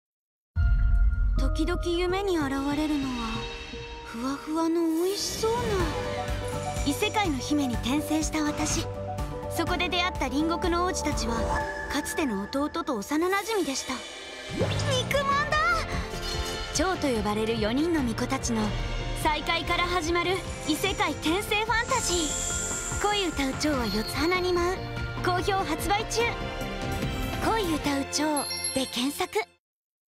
コミックス『恋唄う蝶は四つ花に舞う』TVCM（CV：田村睦心）
「四つ花の蝶」が集うとき、記憶の扉が開く――。

【あらすじ】
弟と幼馴染と共に事故に巻き込まれ 命を落とした高校生の香菜は、
東西南北に位置する四つの国からなる 通称「四つ花」という異世界へと転生する。
前世の記憶を失ったまま西の王国で カーネリア姫として育った彼女は
「四つ花の蝶」と呼ばれる神子に選ばれ、任務で初めて南の王国を訪れる。
そこに待っていた二人の王子。
彼らとの出逢いが 「香菜」の記憶を呼び覚ます――。
新たな世界で舞う蝶たちの、異世界転生ファンタジー。

【書籍情報】
『恋唄う蝶は四つ花に舞う』（ＦＬＯＳ　ＣＯＭＩＣ）
著　：かづか将来
発行：株式会社ＫＡＤＯＫＡＷＡ
コミックス1～2巻　好評発売中！

◆ナレーション：田村睦心